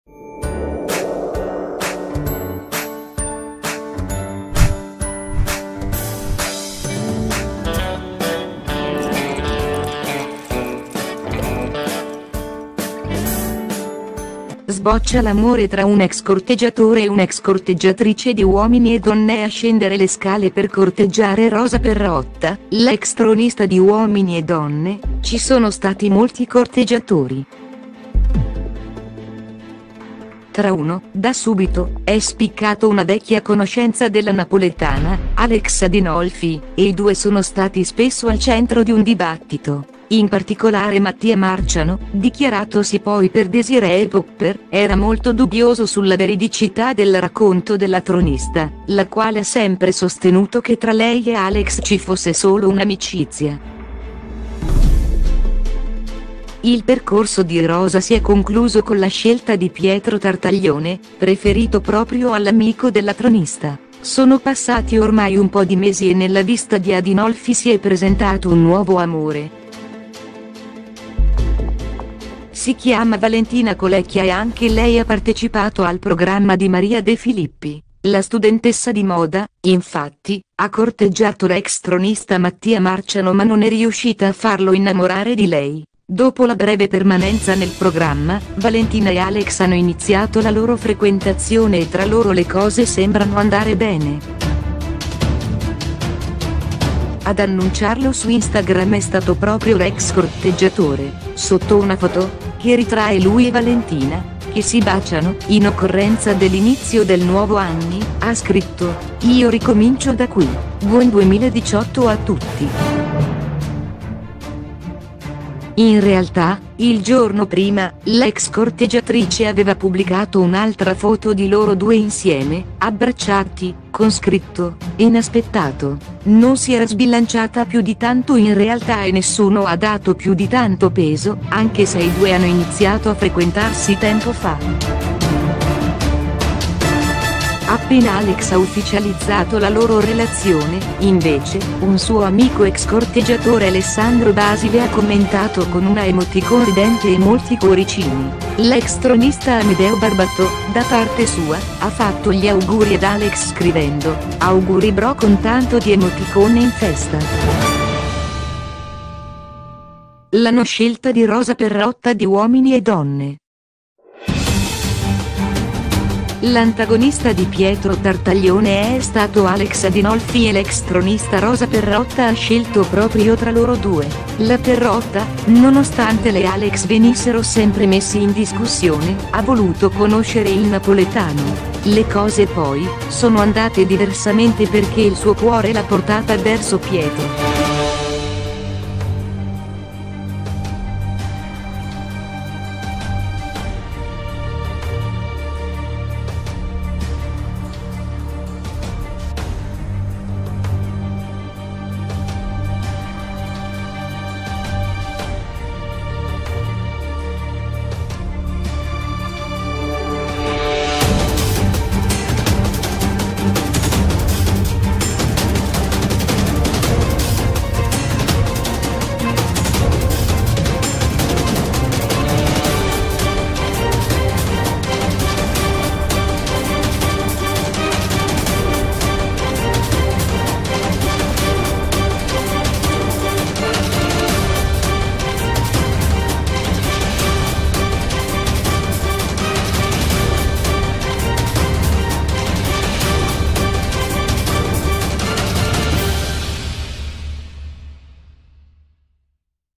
Uomini e donne gossip: è amore tra due ex corteggiatori
Sboccia l’amore tra un ex corteggiatore e un’ ex corteggiatrice di Uomini e DonneA scendere le scale per corteggiare Rosa Perrotta, l’ex tronista di Uomini e Donne, ci sono stati molti corteggiatori.